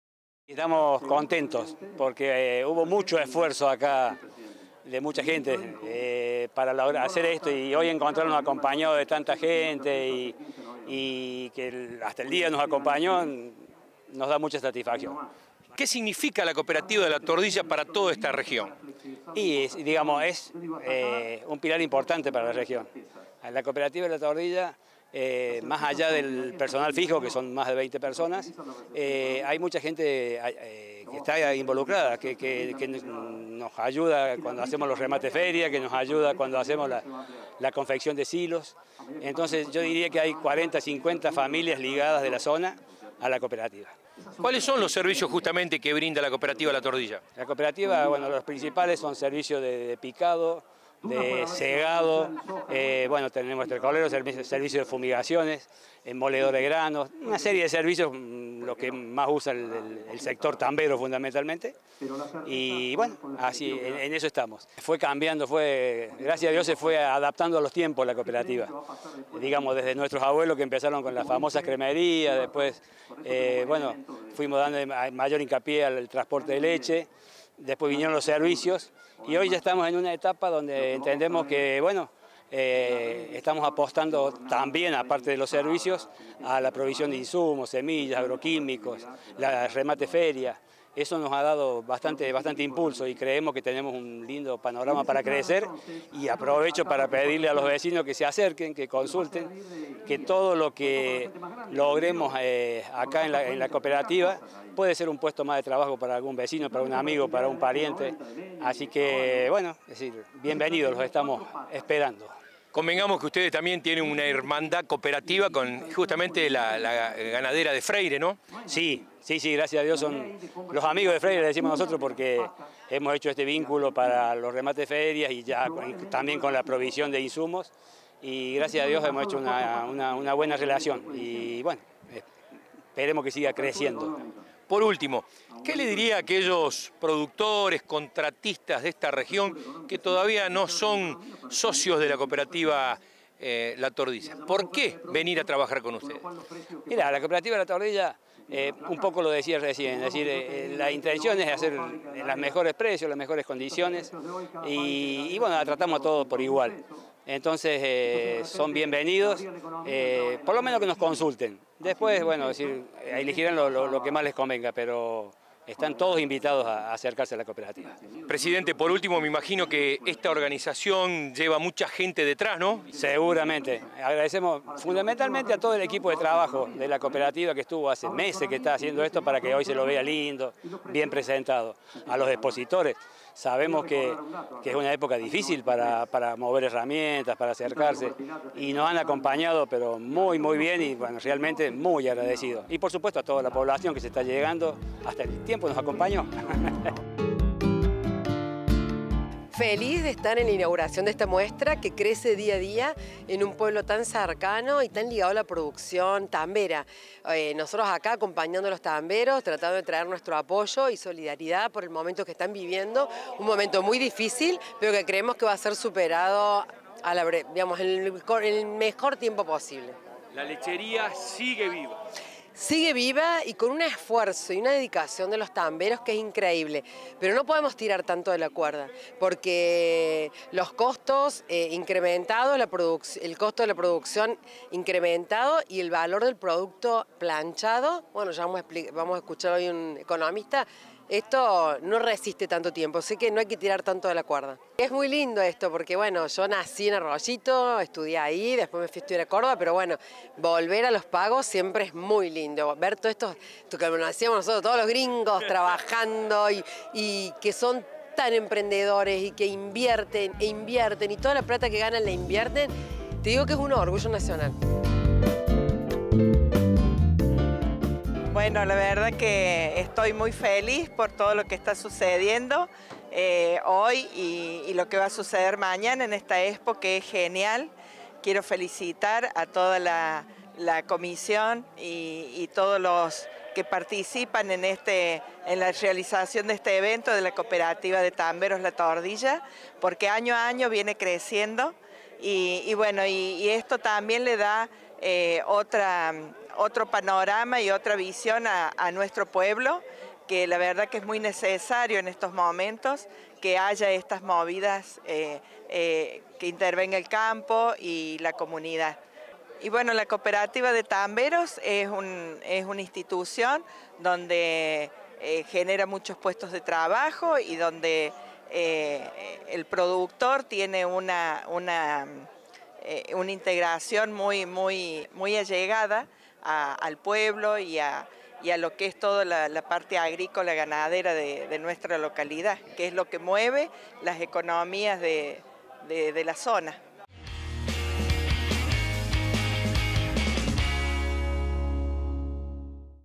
La Tordilla mostró toda su producción en la Expo 2023
Néstor Cipolatti / Cooperativa de Tamberos "La Tordilla" Ltda.
Catalina Boetto / Secretaria de Ganadería de la Provincia de Córdoba
Claudia Bordoni / Intendente de La Tordilla (Córdoba)